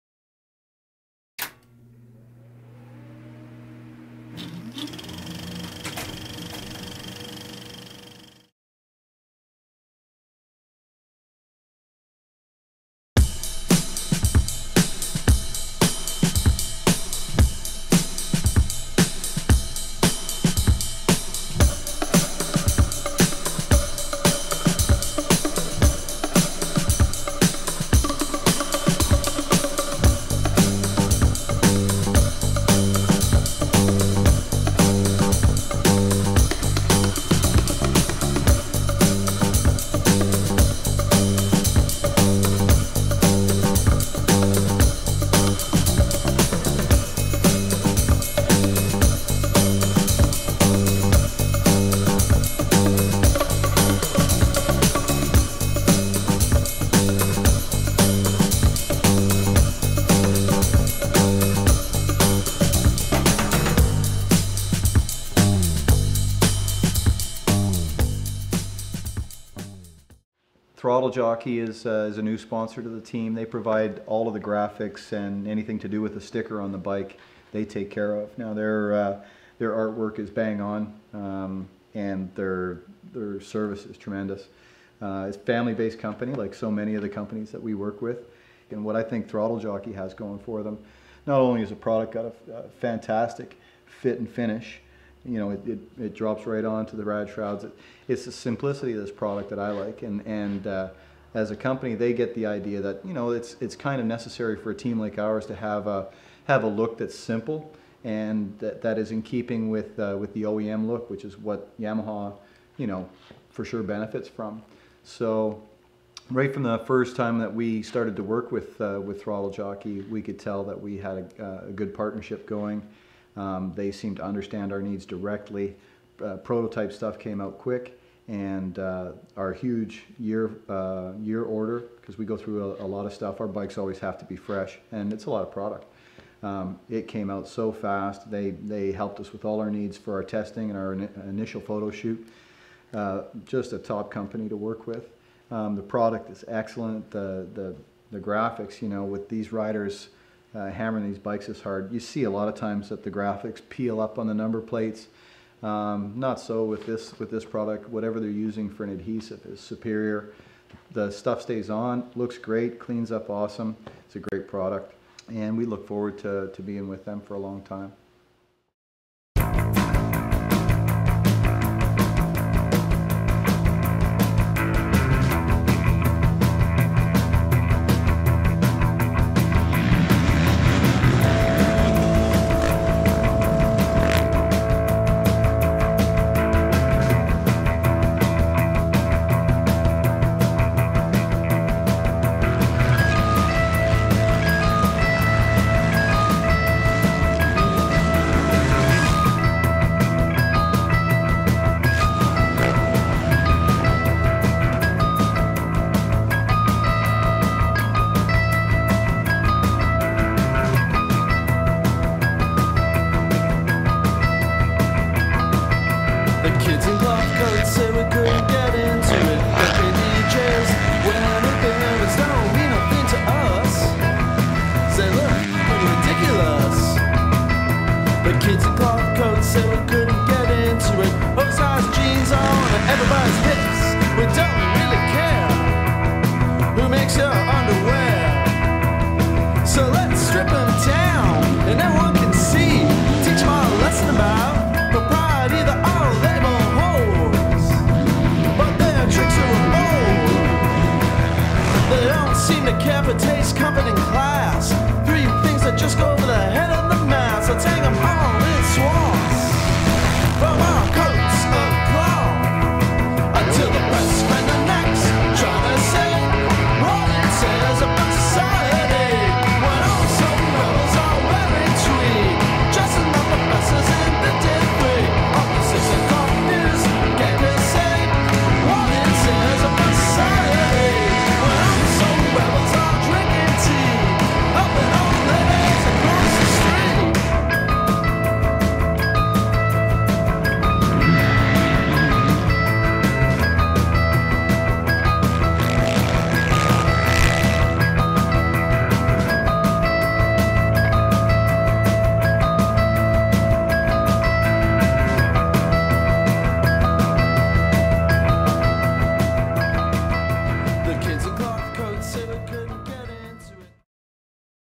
Team Blackfoot Yamaha - Throttle Jockey - Destroyer Films
Check out this promo video of the Blackfoot Yamaha team, and their 2007 season.  Featured riders JSR, Colton Facciotti, Blair Morgan, Jimmy Nelson, and Nick Evennou rip it up from coast to coast on their Throttle Jockey backed Yamaha YZ's.  for more action from the Canadian National Motocross Series...